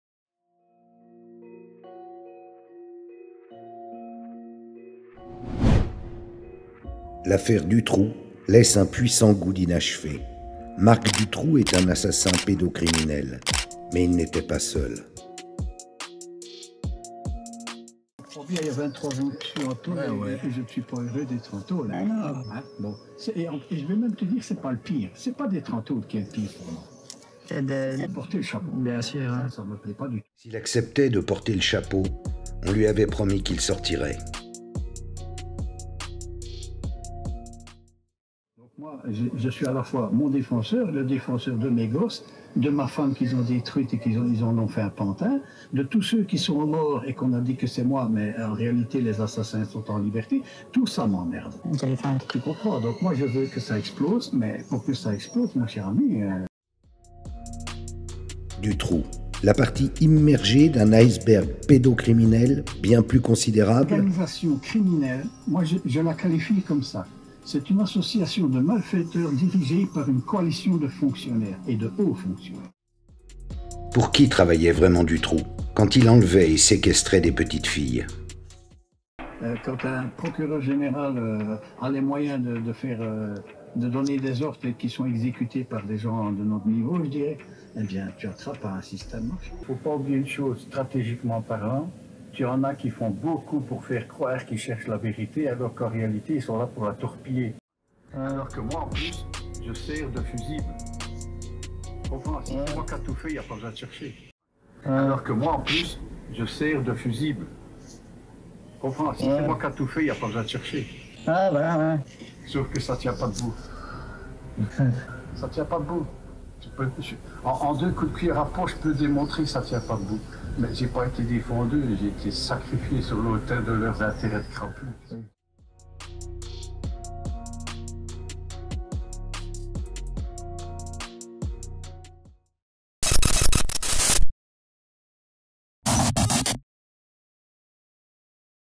Dutroux, la vérité étouffée. Grâce à vous, on va pouvoir produire nos propres docs !
En plus de vous proposer des docs inédits ou imparables, le but de LENVERS-TV est de pouvoir produire les nôtre, qui dévoileront tout, sans censure. Un exemple de ce qu'on veut faire: "Dutroux, la vérité étouffée". Il y a eu plein de docs sur Dutroux, mais jamais aucun qui abordent la question dans sa globalité, celle d’un réseau bien plus vaste que Dutroux lui-même...
Plus que quelques jours...